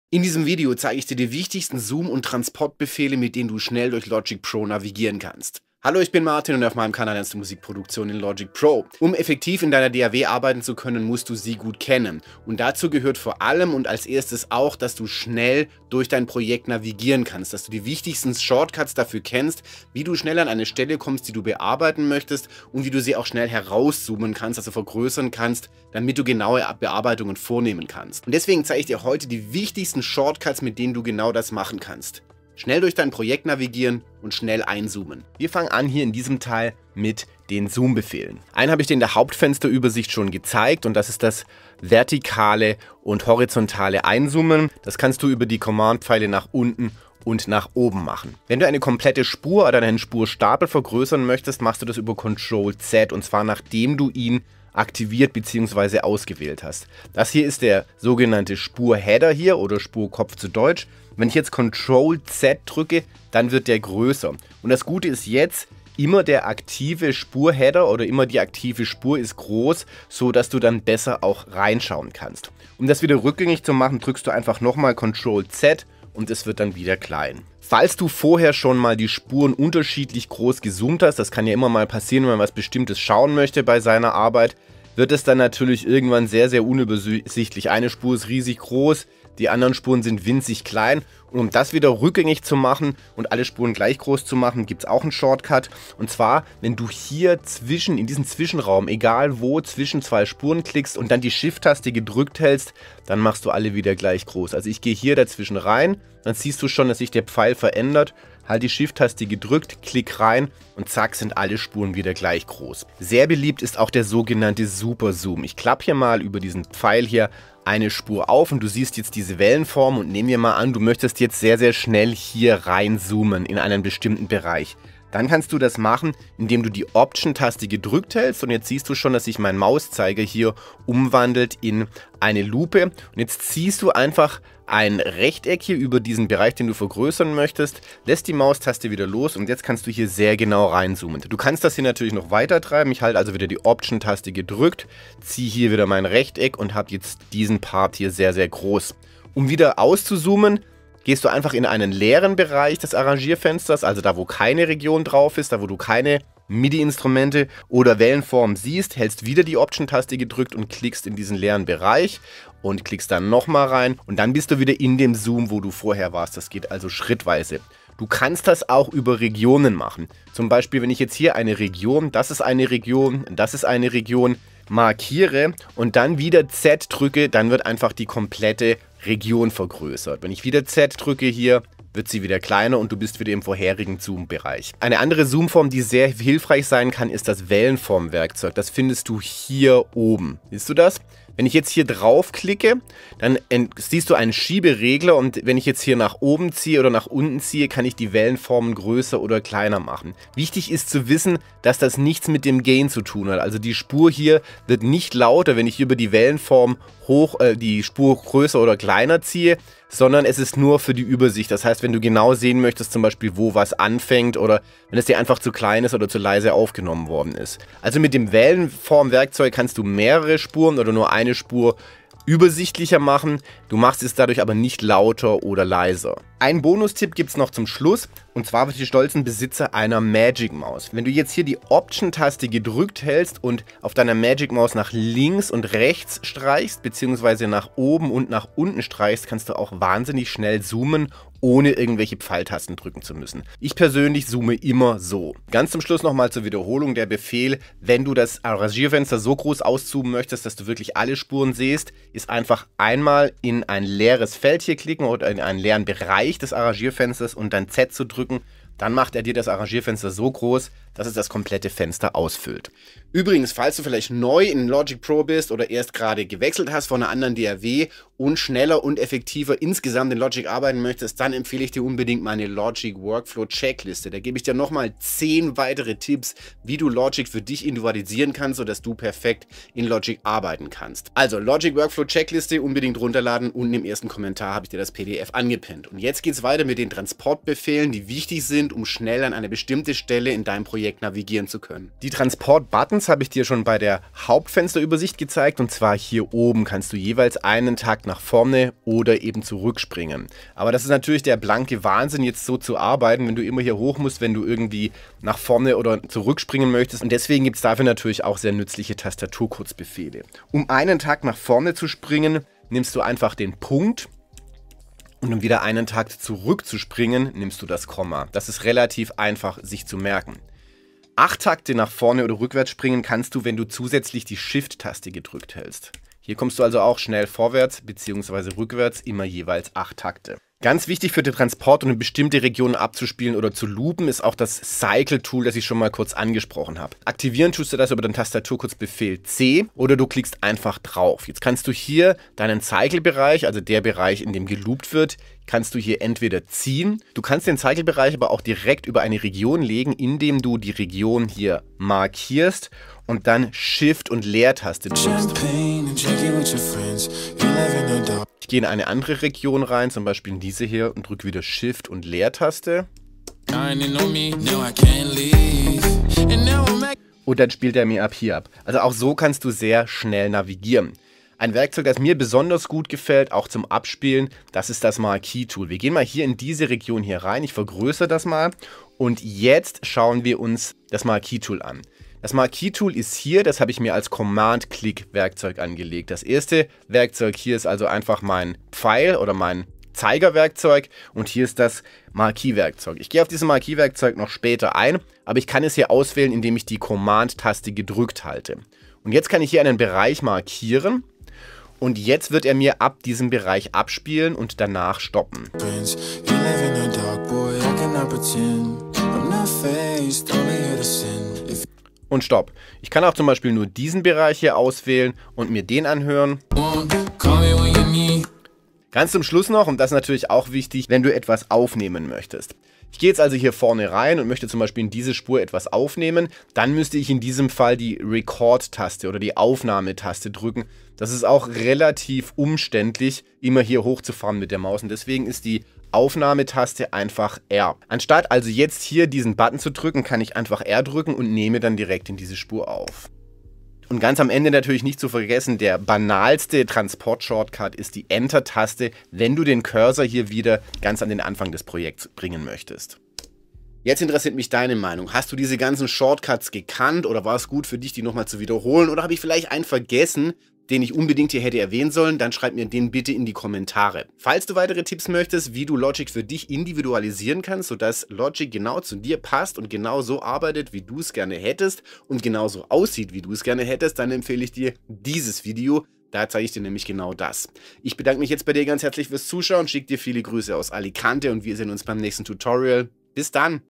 Diese Shortcuts musst du kennen! (Logic Pro X Tutorial deutsch)
Um Effektiv in Logic Pro X zu arbeiten, musst die schnell durch dein Projekt navigieren können. Egal ob du Anfänger oder Profi bist, egal ob du Hip Hop oder Pop Produktionen machst. In diesem Tutorial auf deutsch zeigt dir Martin Wolfinger die wichtigsten Navigations- und Zoom Shortcuts, die dir dabei helfen können.